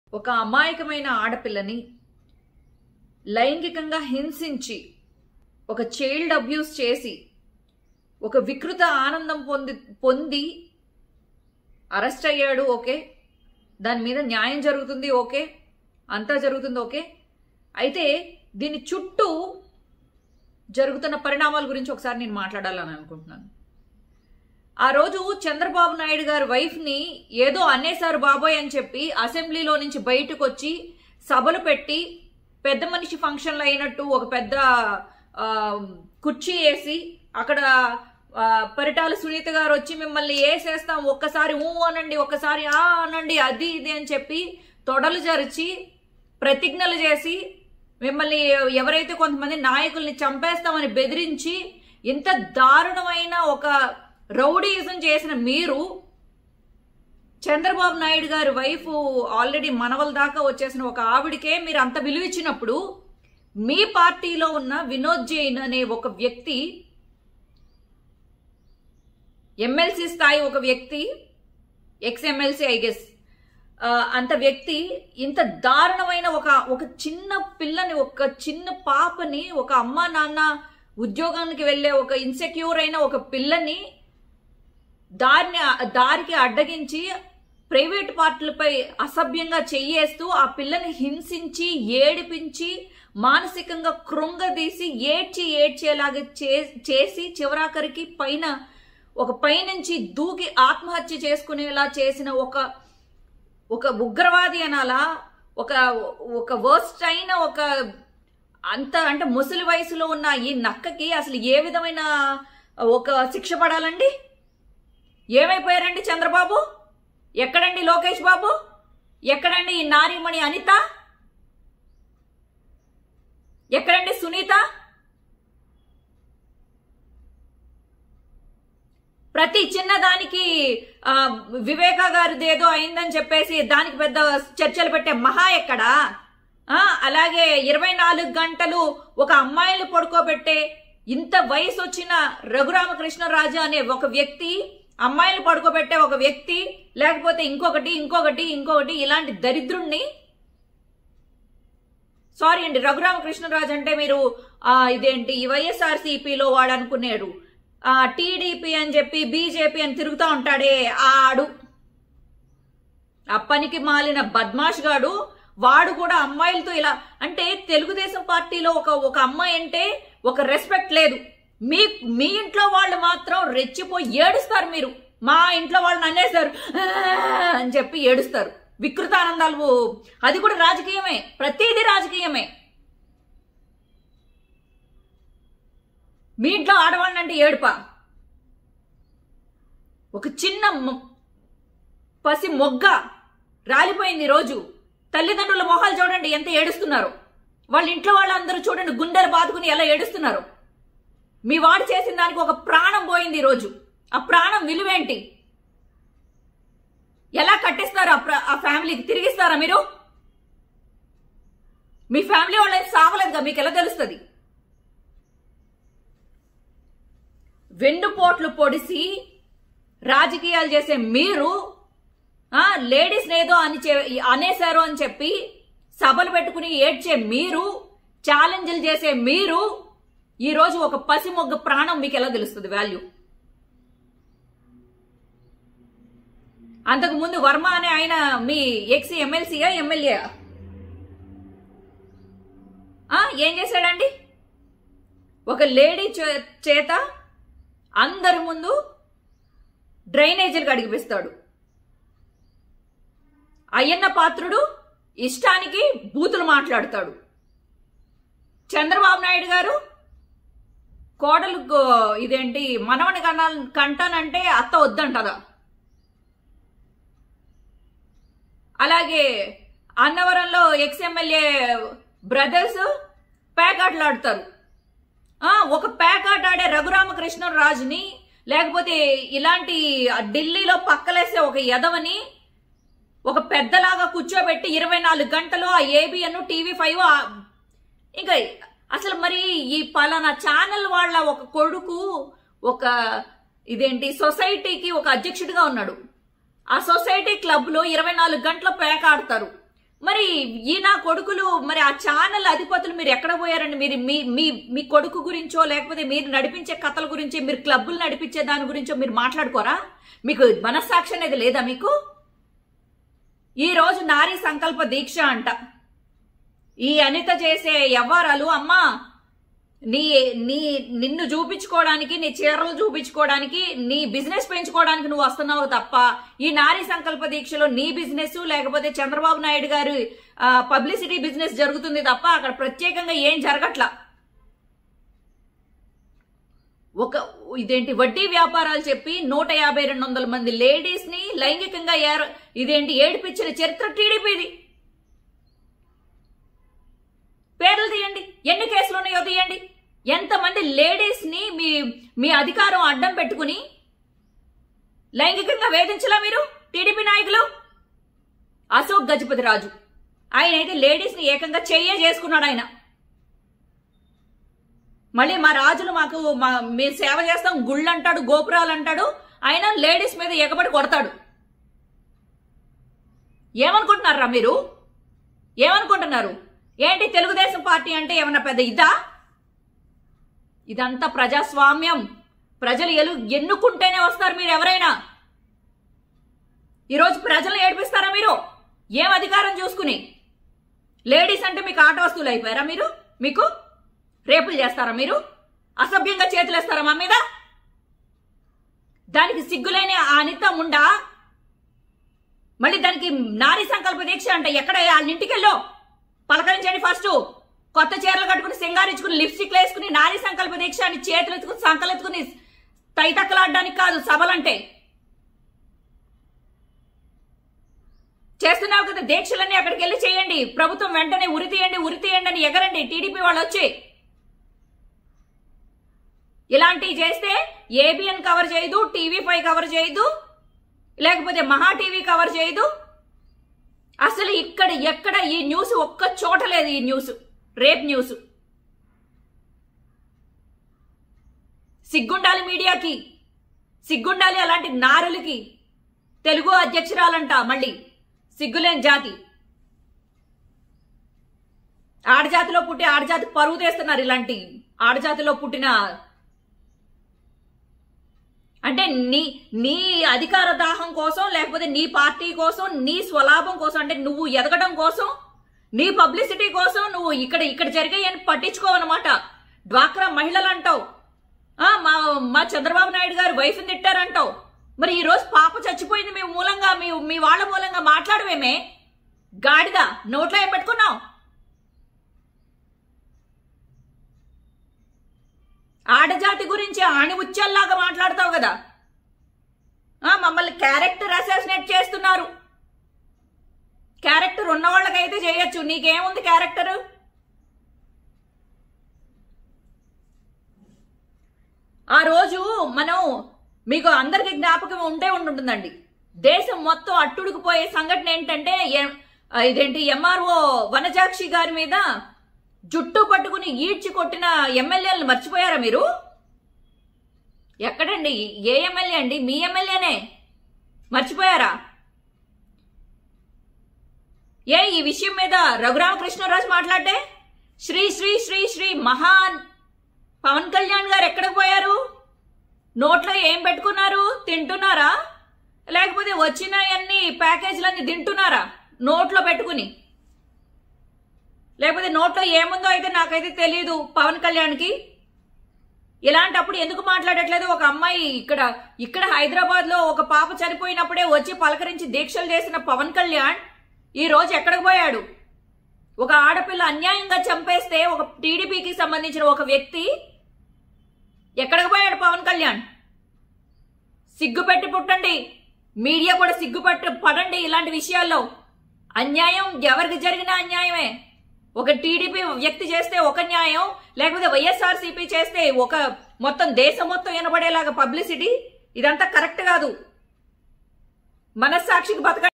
Sri Reddy Live | Sri Reddy Serious on Vijayawada Girl Issue TDP | Anitha || MB Media Telugu
Sri Reddy Hot comments On TDP # Sri Reddy comments Anita Kankipadu News AP TDP Sri reddy comments on vangalapudi anitha